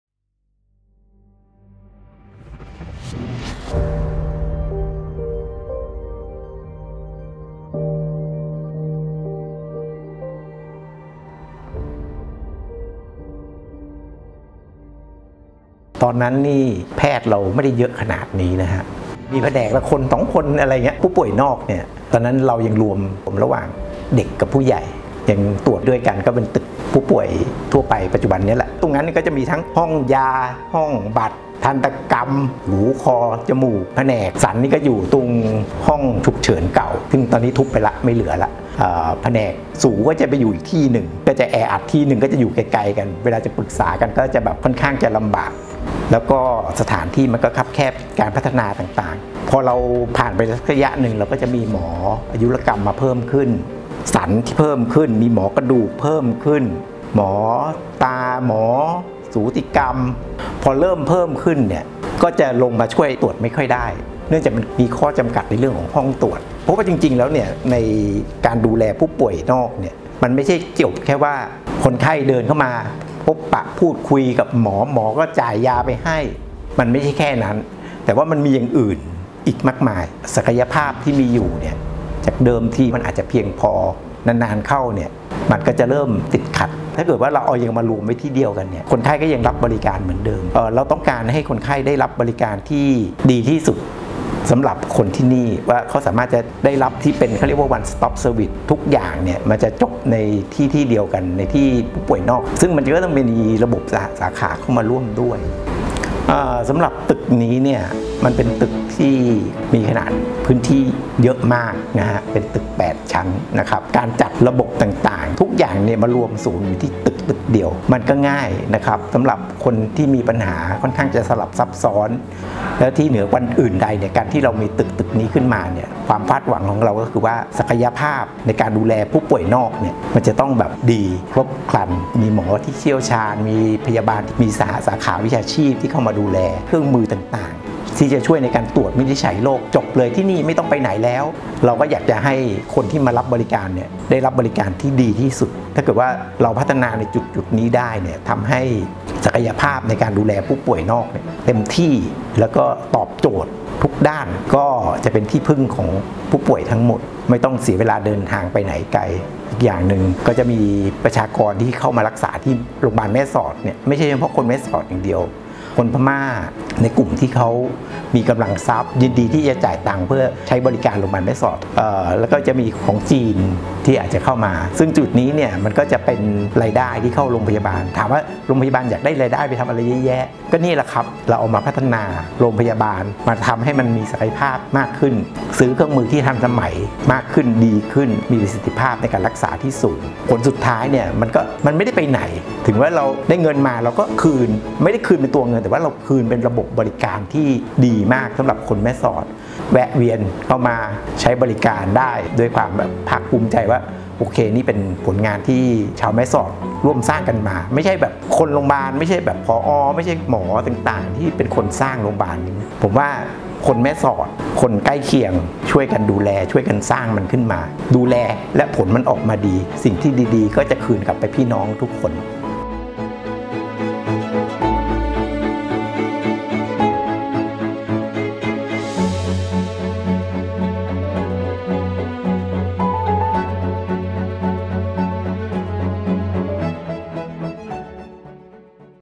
กว่าจะเป็นอาคารผู้ป่วยนอก
วิดีโอเนื่องในพิธีเปิดอาคารกรมสมเด็จพระเทพรัตนราชสุดา (อาคารผู้ป่วยนอก 8 ชั้น)